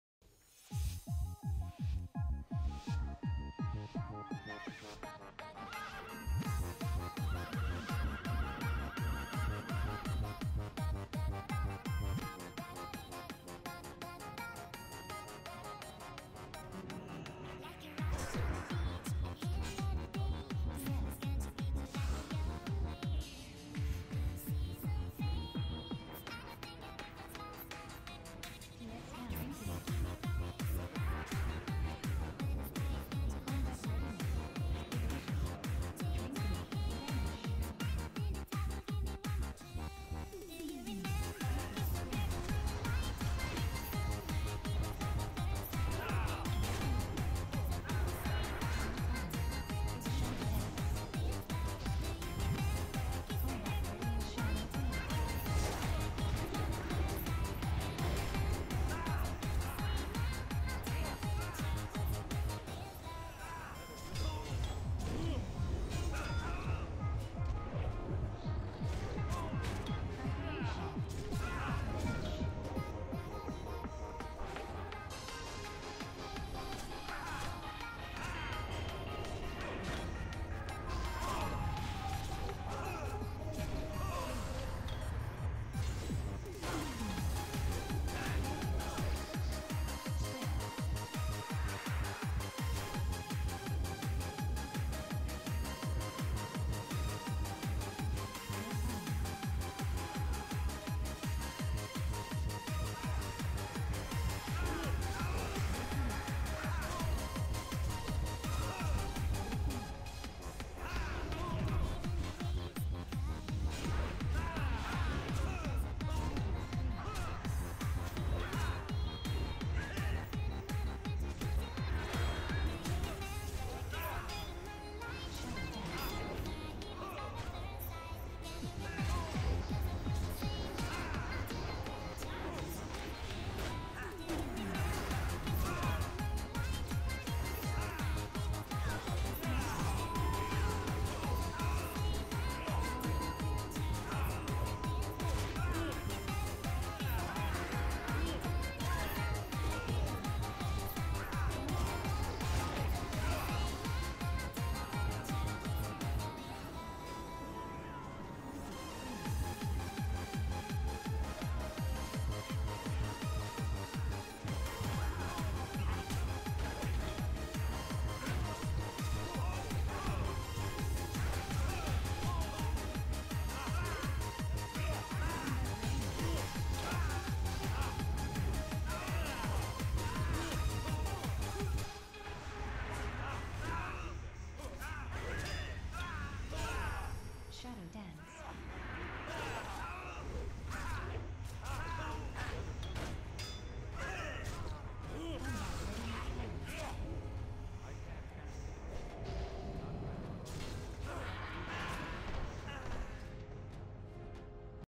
Warrior and Rogue vs Priest and Rogue
This is a video of how my partner and I roll when facing priests. Not the cleanest fight but still I tried my best to limit the amount of healing the priest did, and imo it worked out to my advantage